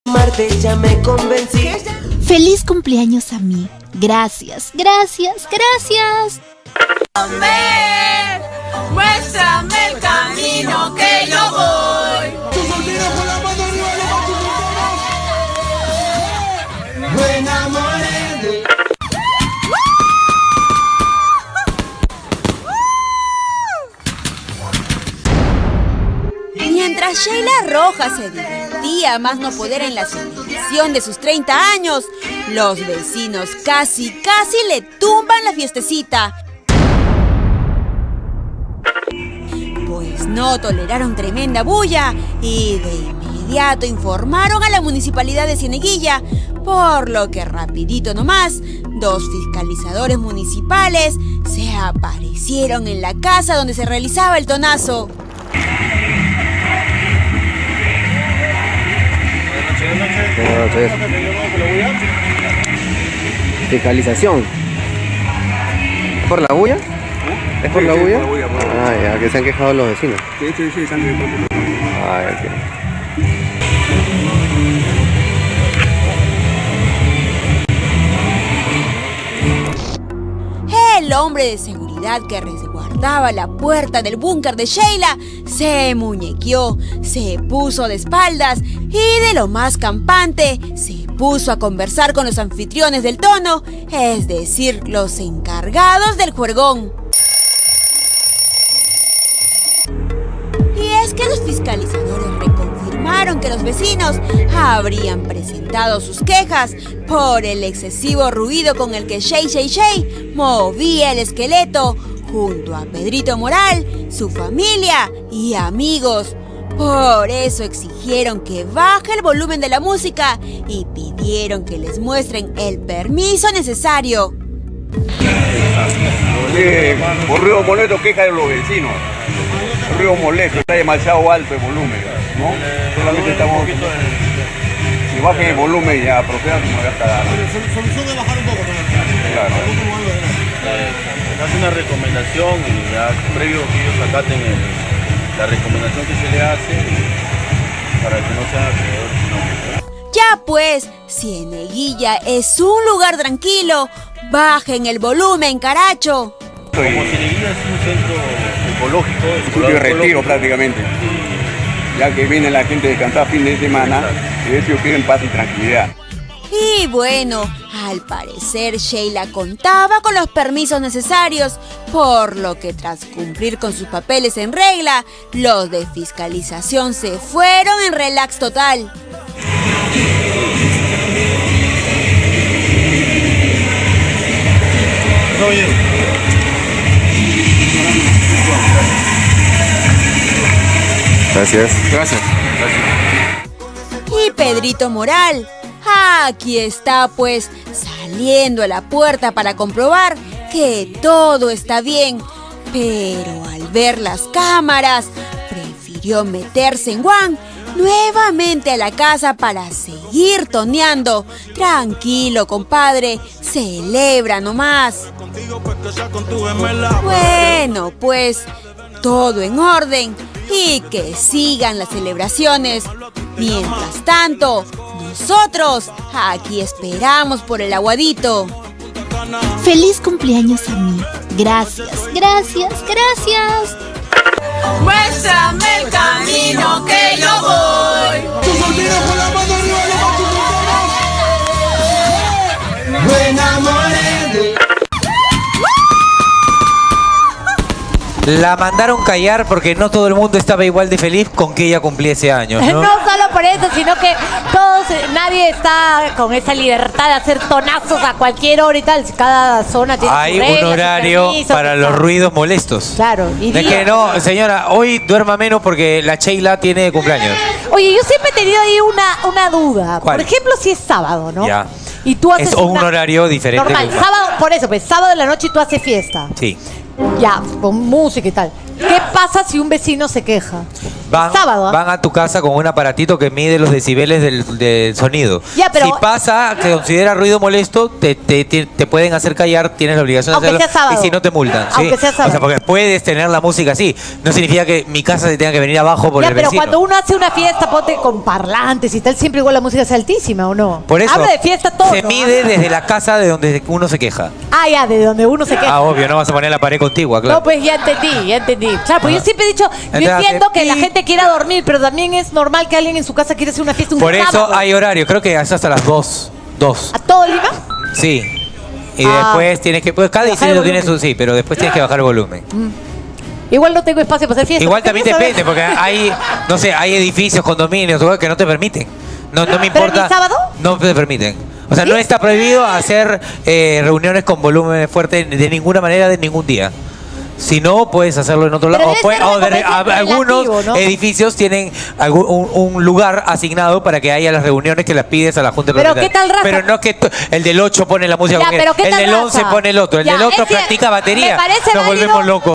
FISCALIZADORES LE JALAN LAS OREJAS A SHEYLA ROJAS POR PONER MUSICA A FULL VOLUMEN
le aguaron la fiesta a sheyla rojas, fiscalizadores le piden bajar volumen de su musica.